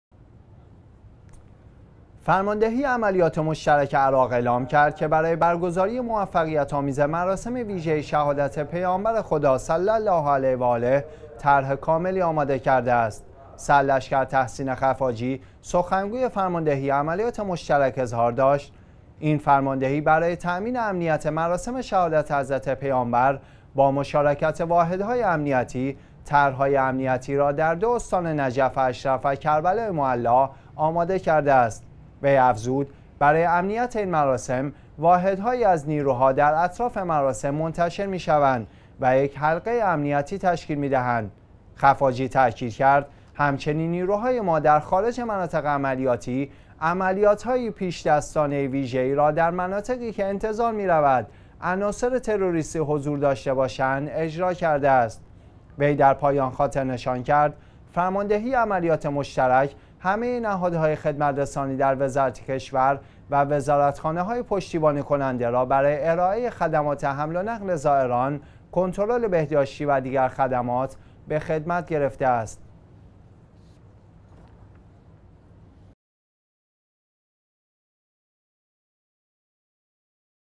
تامین امنیت مراسم شهادت پیامبر اسلام در نجف و کربلا
فرماندهی عملیات مشترک عراق اعلام کرد که برای برگزاری موفقیت آمیز مراسم ویژه شهادت پیامبر خدا صلی علیه و آله، طرح کاملی آماده کرده است.
سرلشکر «تحسین خفاجی» سخنگوی فرماندهی عملیات مشترک اظهار داشت: این فرماندهی برای تامین امنیت مراسم شهادت حضرت پیامبر، با مشارکت واحدهای امنیتی، طرح های امنیتی را در دو استان نجف اشرف و کربلای معلی آماده کرده است.
وی افزود: برای امنیت این مراسم، واحدهایی از نیروها در اطراف مراسم منتشر می شوند و یک حلقه امنیتی تشکیل می دهند.
خفاجی تاکید کرد: همچنین نیروهای ما در خارج مناطق عملیاتی، عملیات های پیش دستانه ویژه ای را در مناطقی که انتظار می رود عناصر تروریستی حضور داشته باشند اجرا کرده است.
وی در پایان خاطرنشان کرد: فرماندهی عملیات مشترک، همه نهادهای خدمات رسانی در وزارت کشور و وزارت خانه های پشتیبانی کننده را برای ارائه خدمات حمل و نقل زائرین، کنترل بهداشتی و دیگر خدمات، به خدمت گرفته است.